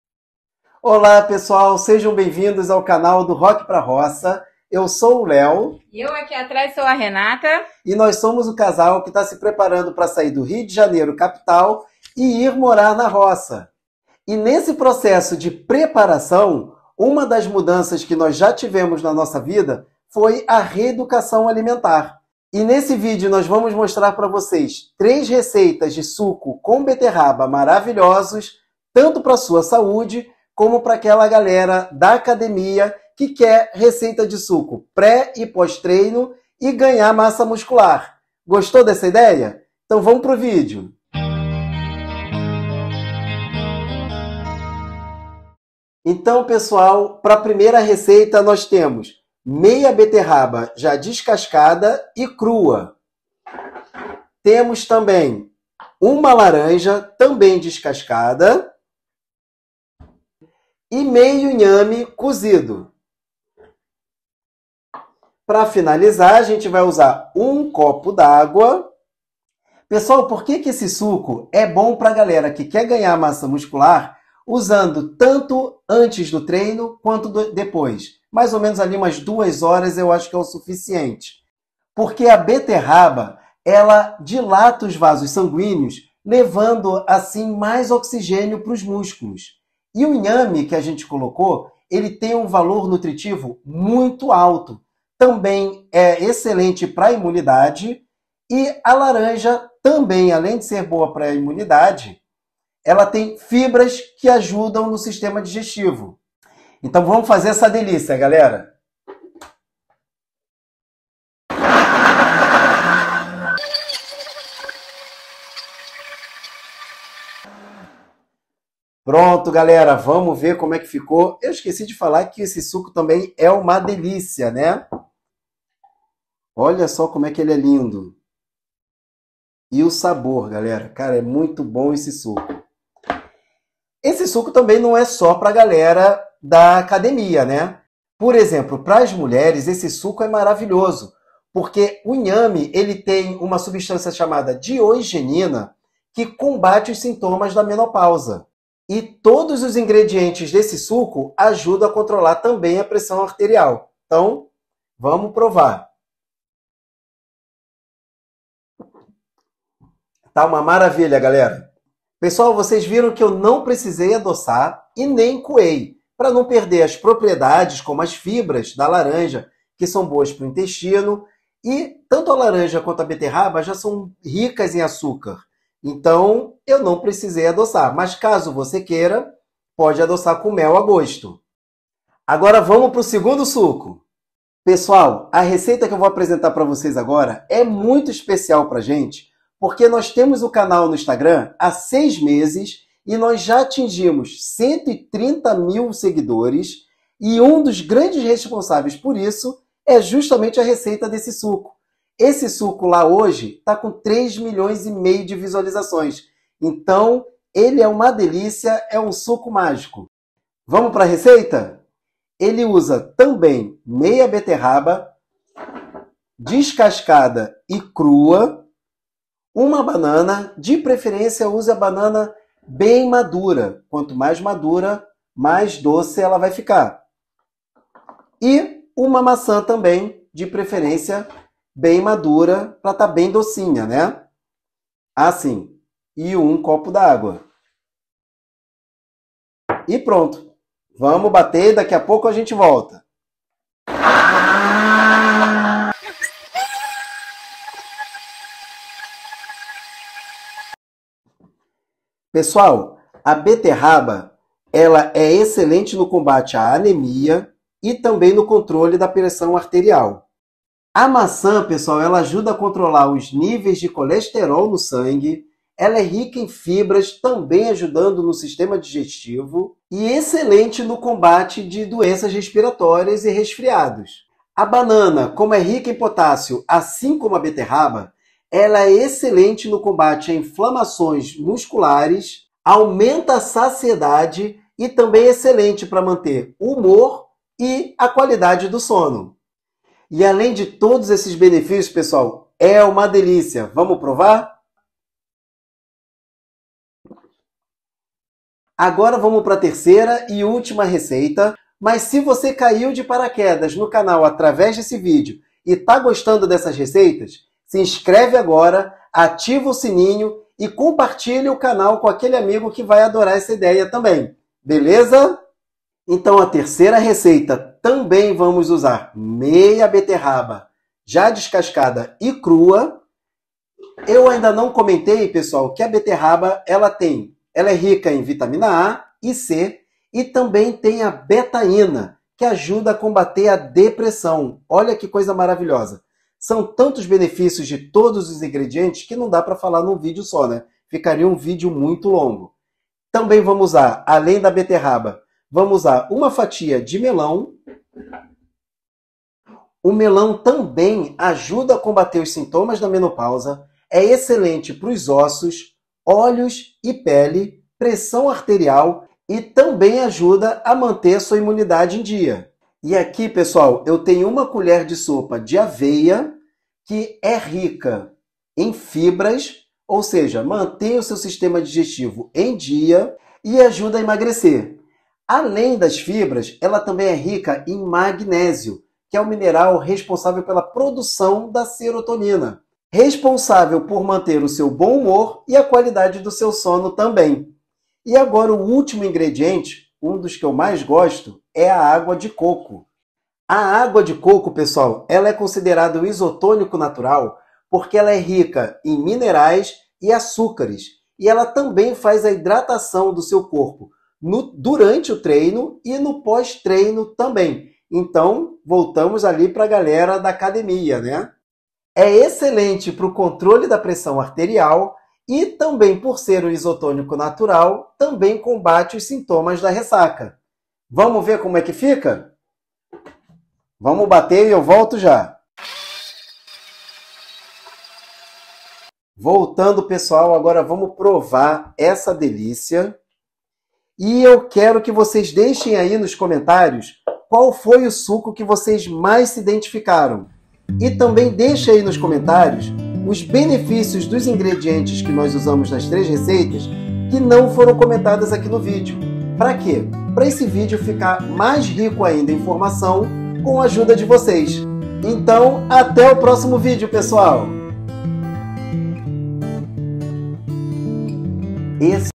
Beterraba - 3 Sucos Pré-treino e para sua Imunidade. 🍀😍
Hoje vamos fazer 3 Receitas de Sucos Perfeitos para o Pré e Pós treino e Ganho de Massa Muscular. 😱

Os benefícios vão muito além!

Falamos um pouco sobre os benefícios de cada Receita durante o vídeo.

Tenho certeza que você vai adorar 🥰😋

😍 SUCO 1

✅ INGREDIENTES:

- 1/2 beterraba crua
- 1 laranja
- 1/2 inhame cozido
- 1 copo d'água

😍 SUCO 2

✅ INGREDIENTES:

- 1/2 beterraba crua
- 1 banana bem madura
- 1 maçã
- 1 copo d'água

😍 SUCO 3

✅ INGREDIENTES:

- 1/2 beterraba crua
- 1 fatia de melão
- 1 colher de sopa de aveia
- 1 copo de água de coco

✅ PREPARO:

Bata tudo no liquidificador.

🚨 O ideal é não adoçar.

O sabor é uma delícia! 😋

Gostou da ideia? 🍀😍

Curta e Compartilhe com aquele amigo que vai adorar essa ideia! 😁